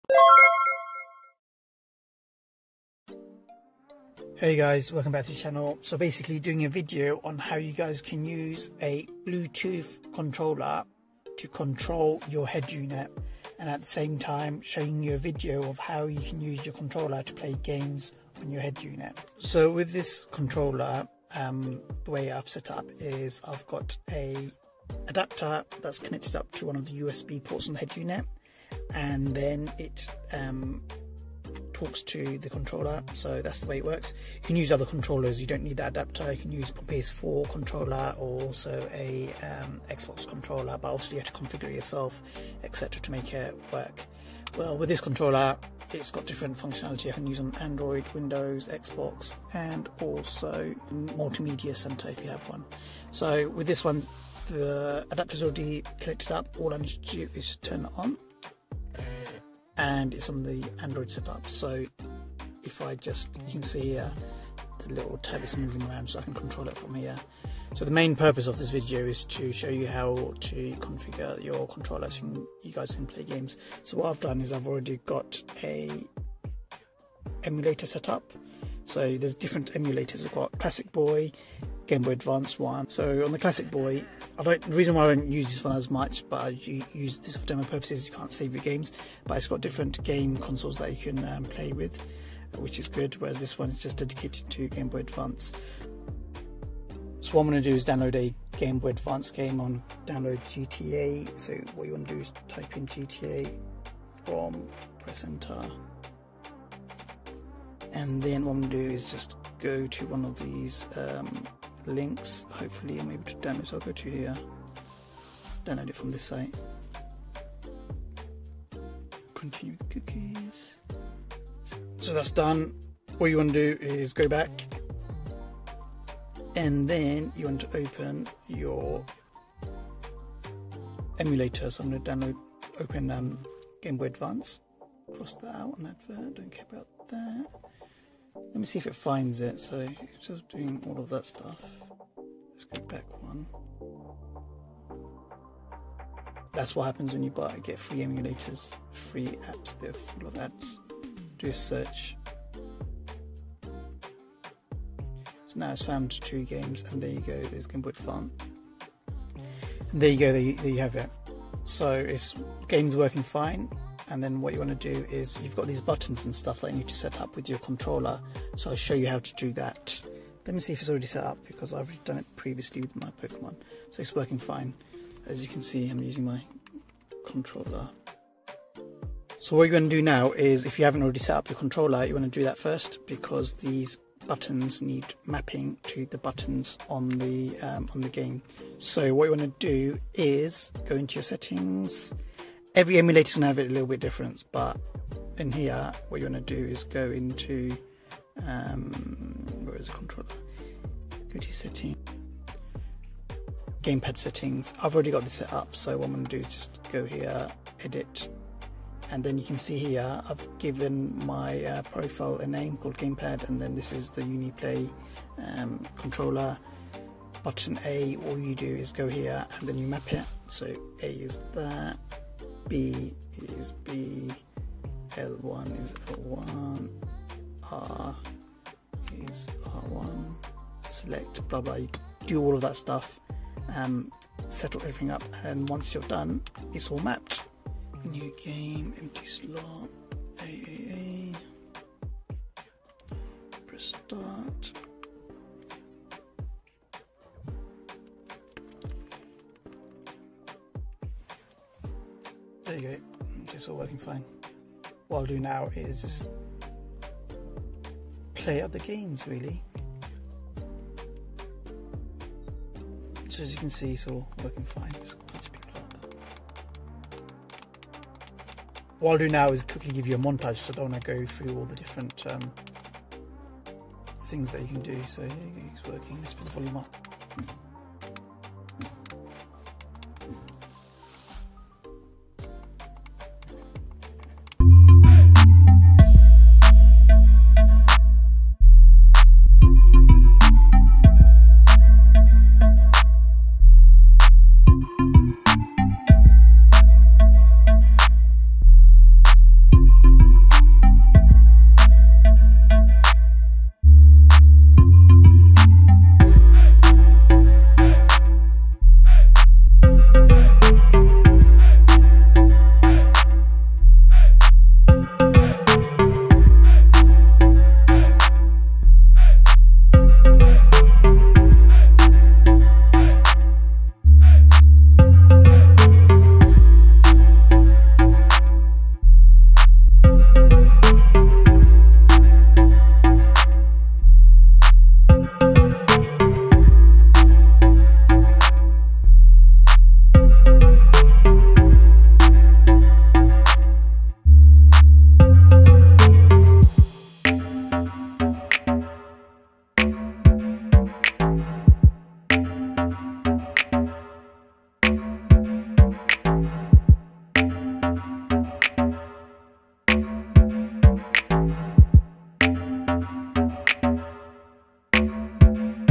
Setting up gaming Emulators and controller on a Android Headunit
A tutorial on how to set up a controller and emulators to play games on an Android Head Unit.

I used a Bluetooth controller with my Android Headunit & it worked pretty well.